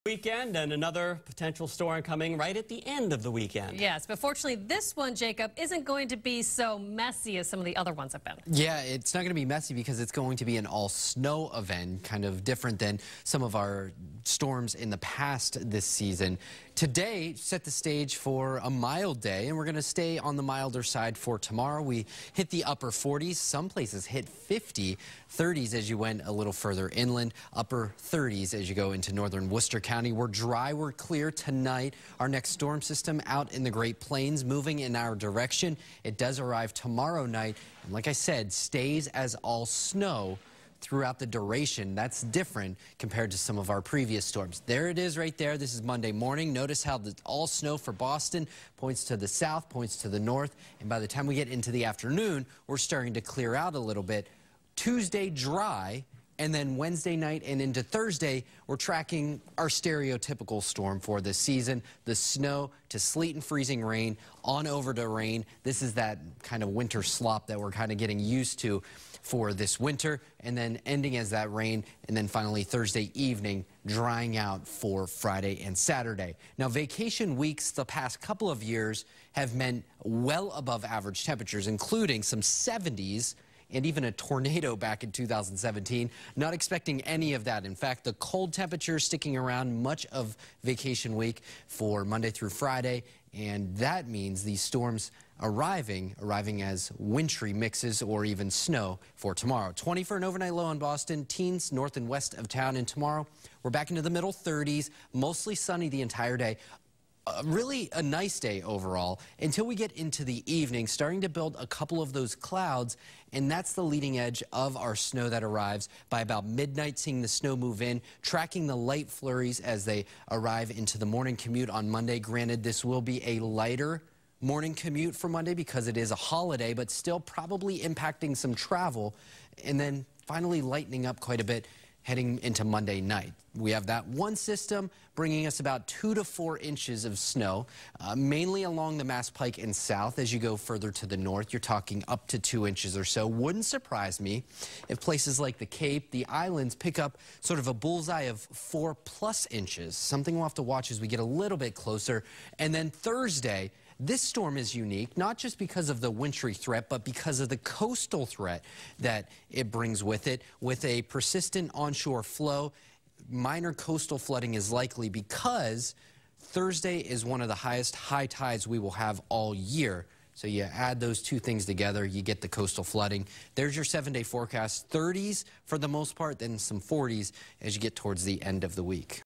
WBZ Weather Forecast
Jacob Wycoff has your latest weather forecast.